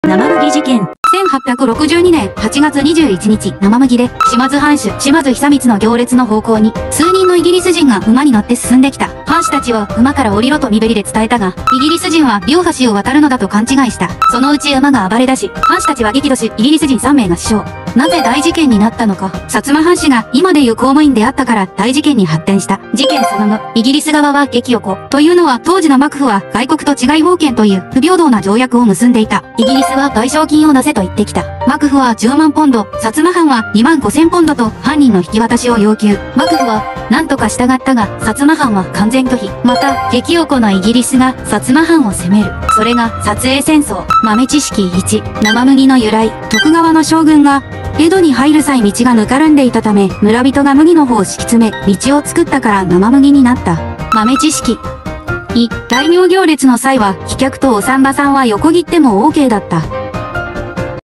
生麦事件 島津久光 薩摩藩 イギリス人 賠償金 namamugiincident shorts
この事件は、海外の国をあまり知らない頃(中にはいましたが)。だから、このような事件が起きてしまった。その後、日本は海外の国が、どれだけ強いかを思い知る。